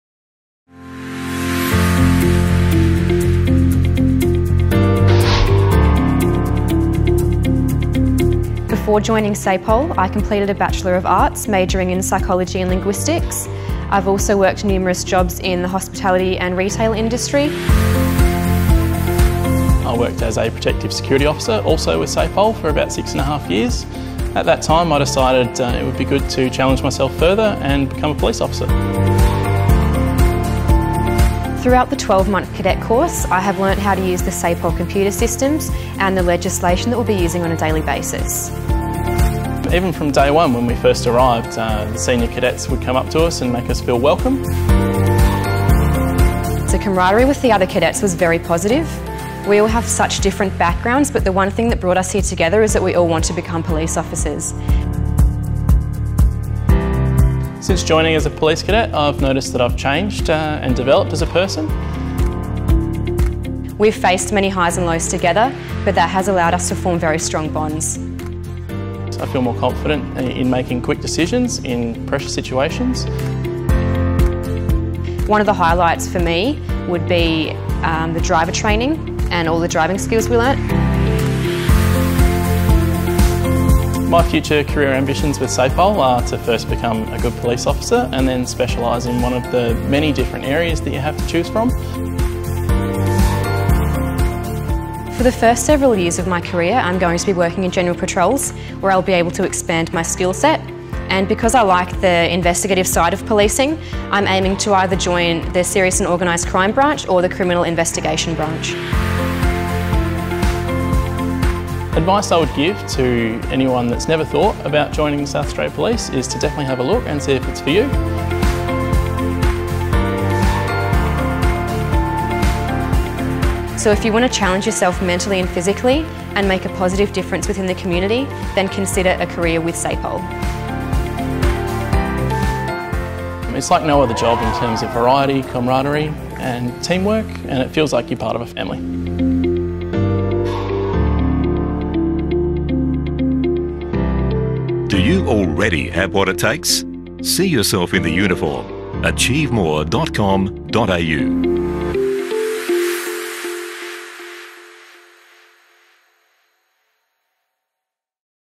Introducing 2 of our new graduate cadets - Jayme and Sarah
SA Police welcomed 40 new officers to the job, with two cadet courses graduating during a ceremony at the Academy on Wednesday 10 January.

For more information about joining SA Police – including the option of working as a Protective Security Officer – please visit our recruiting